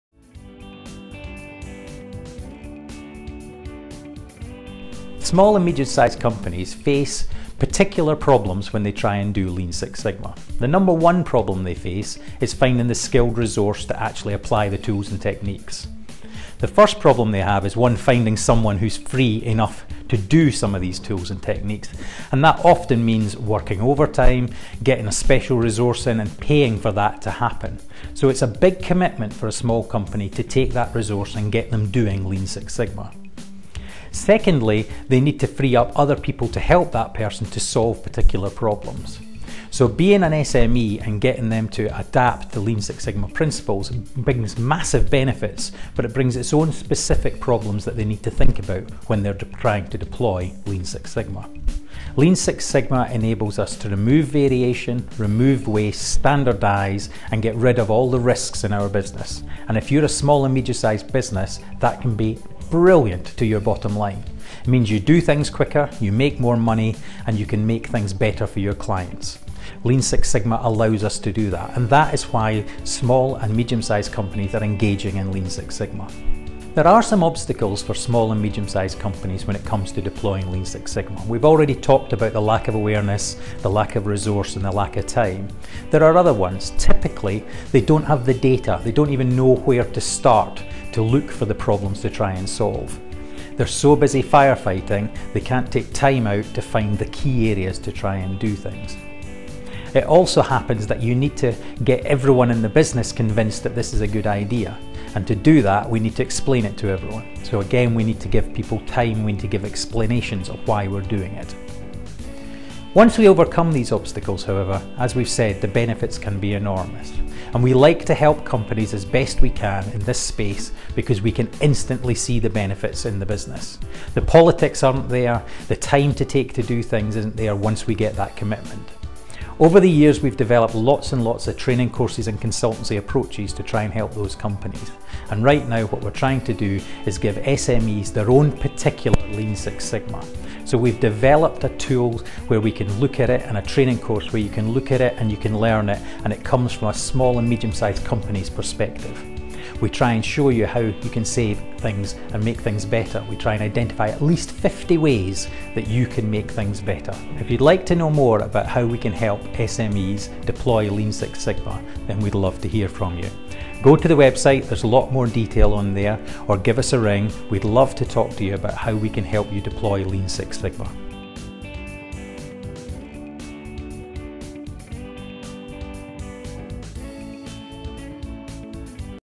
Why SMEs should consider Lean Six Sigma
Lean Six Sigma is often, incorrectly, considered the domain of big business. However, we explain how LSS for SMEs can be delivered more quickly and efficiently to meet their specific needs.